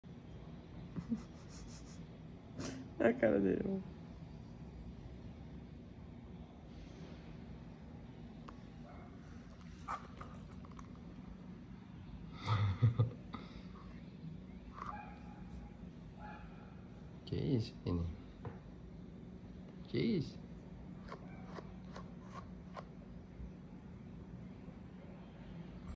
Brushing a Well-Mannered Shih Tzu || ViralHog
Occurred on November 17, 2022 / Santos, São Paulo, Brazil

A shih tzu dog called Brownie at home being brushed on her lap.